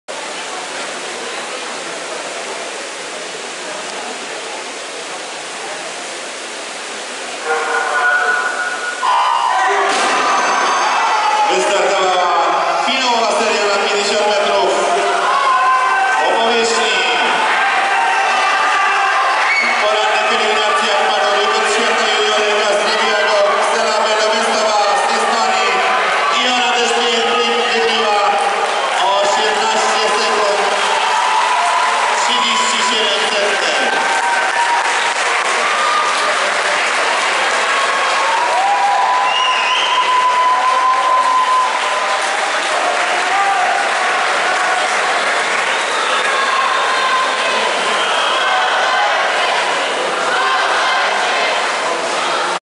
2013 Szczecin 25 TUGCEM AKGUN 50 SF
CMAS 2013 Szczecin Finswimming European Junior Championship / CMAS 2013 Szczecin Paletliyüzme Avrupa Gençler Şampiyonası